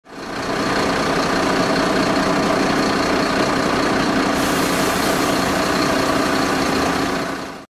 Neuer GW-Wasserrettung BF Mainz FW 2 Innenstadt
Wiesbaden/Mainz - Gegen 21:00 Uhr wurde die Feuerwehr auf die Theodor-Heuss-Brücke gerufen. Eine Person ist von der Brücke aus in den Rhein gesprungen. Zahlreiche Einsatzkräfte suchten ca. 1 Stunde das Rheinufer ab. Nach ca. 1,5 Stunden war der Einsatz beendet.

©RheinMainTaunus112/Wiesbaden - 01/2018 - Mainz-Amöneburg (Wiesbaden) - Sony FDR-AX53 - 4K

Distanzierung: folgt!